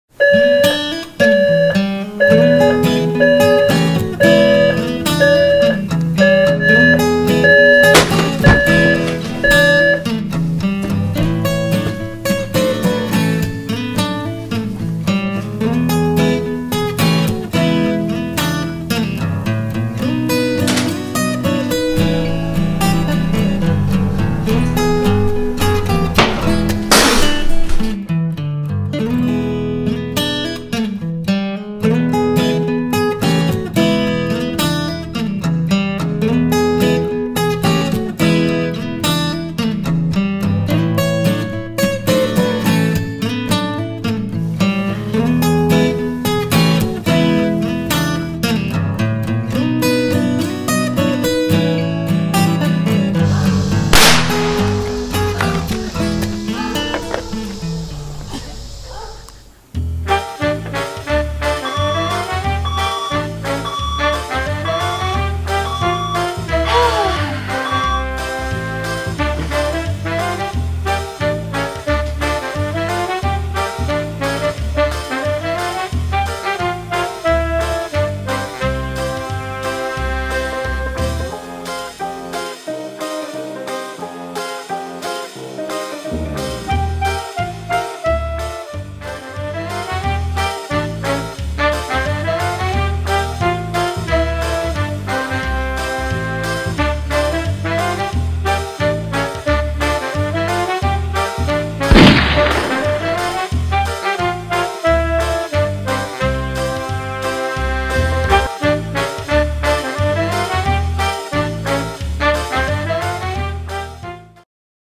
ModCloth Style Short - Have a ModCloth Day
A style short by Sarah Hinson for ModCloth's style short contest.